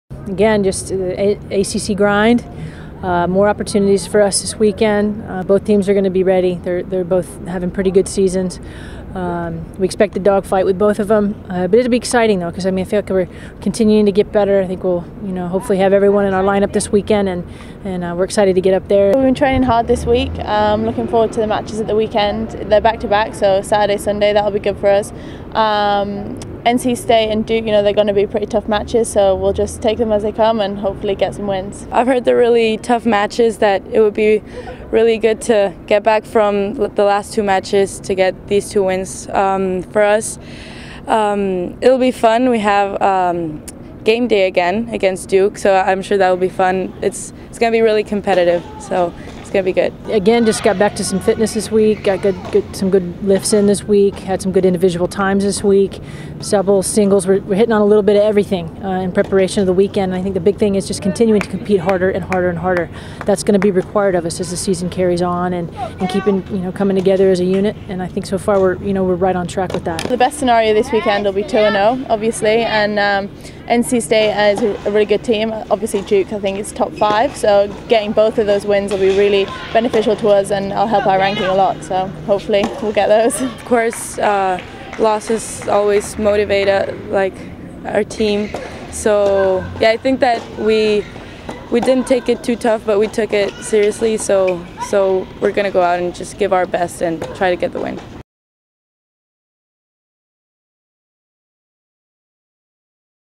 Seminoles Hit Tobacco Road for Pair of Matches
Courtesy The Florida State women's tennis team takes on North Carolina State and Duke this weekend.